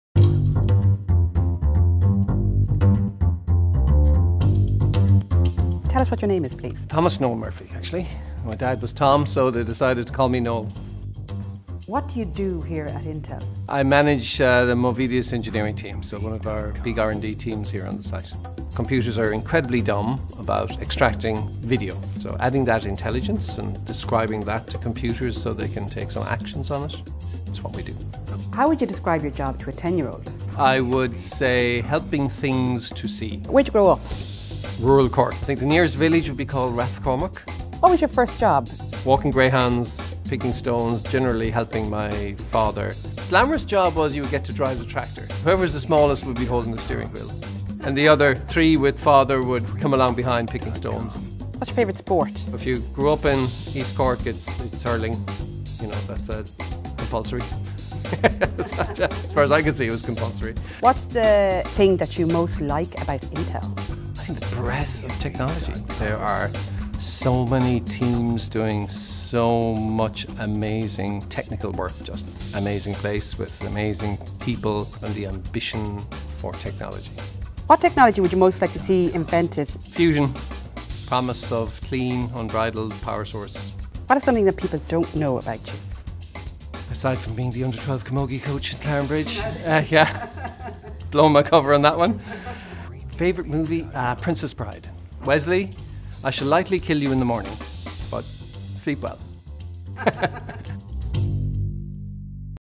Noel Murphy - Intel Ireland Leader series
How would Noel Murphy describe his job to a 10 year old and what’s his favourite film?

These are just two of the questions we asked Noel Murphy, Vice President of Intel’s AIPG group and Director of Engineering, Intel Movidius, when we caught up with him recently for the first in a series of Intel Ireland Leader videos.

Over the next 5 weeks we will bring you a candid chat with some of Intel Ireland’s business leaders as we find out everything from what they wanted to be when they grew up to their favourite pastimes.